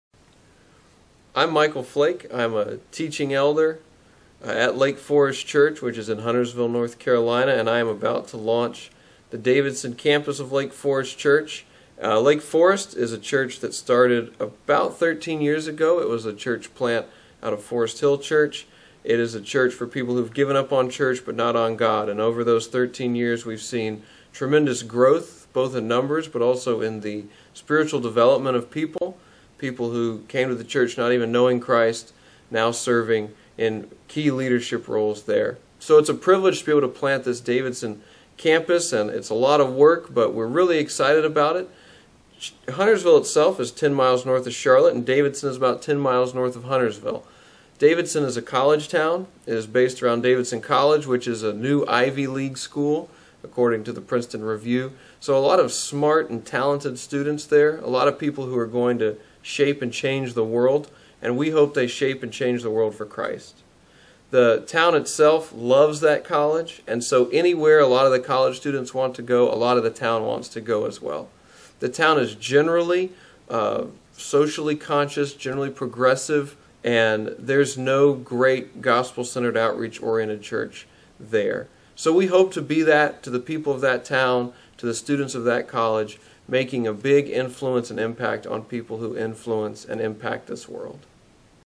EPC church plant - Huntersville, NC
An interview with Micheal Flake about the Lake Forest Church campus at Huntersville, SC.